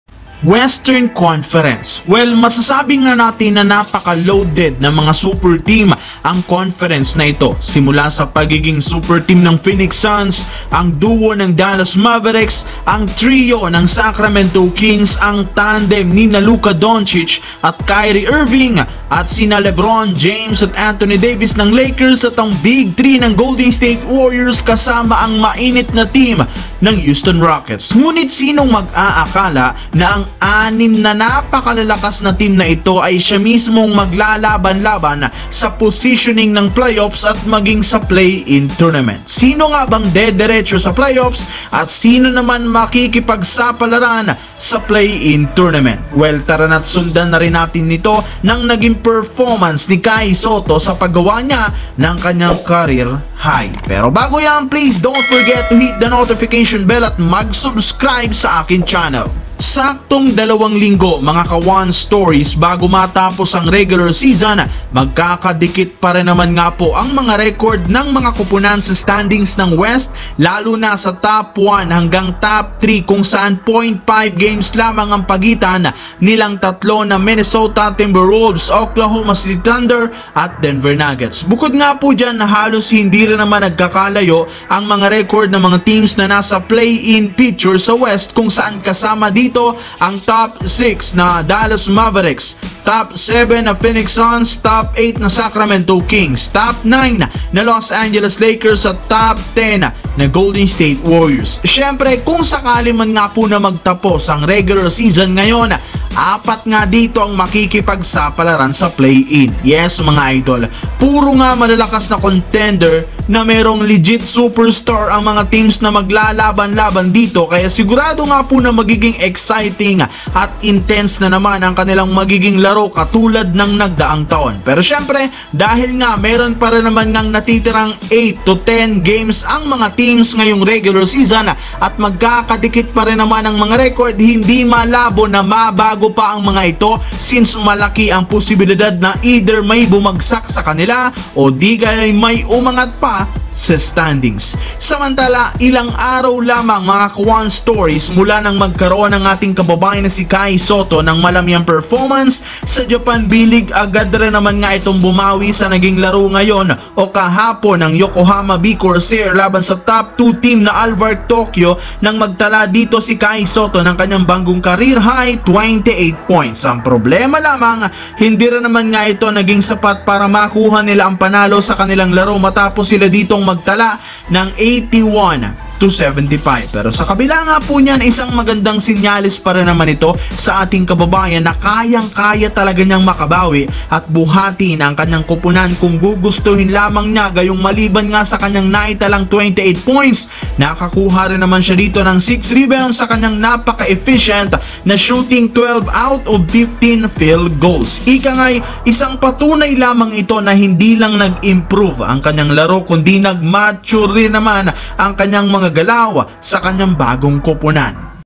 LAKERS GSW SUNS KINGS "SUPERTEAM" nasa LABANAN sa PLAY-IN | Grabe "BINAGO" LUMAKAS si KAI SOTTO
Halina't pag usapan natin ngayon ang ating kababayan na si Kai Sotto.